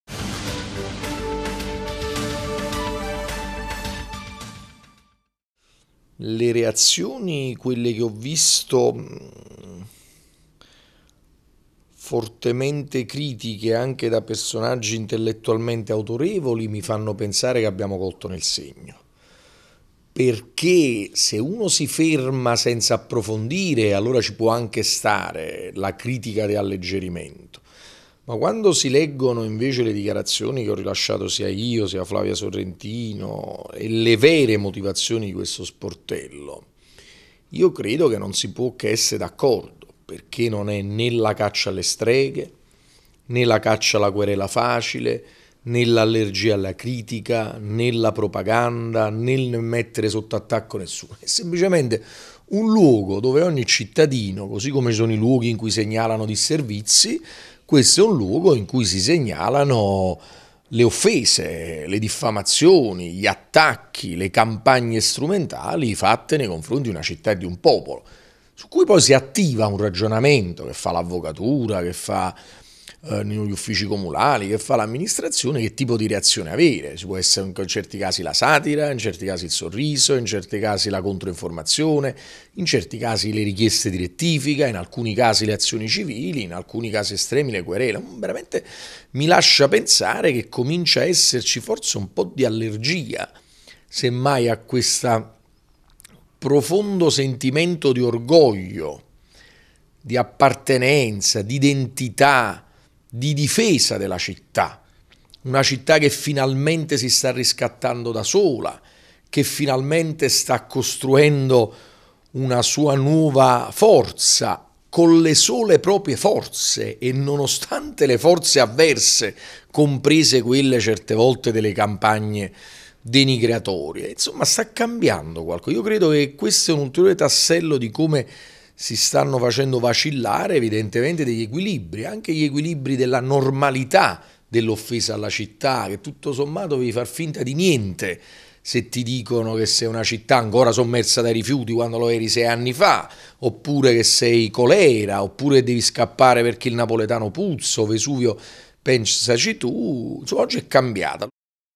Reazioni allo sportello "Difendi la città", sindaco: non sarà più "normale" offendere Napoli
Le dichiarazioni del sindaco Luigi de Magistris sulle reazioni apparse anche sulla stampa nazionale dopo l’apertura dello sportello web "Difendi la città".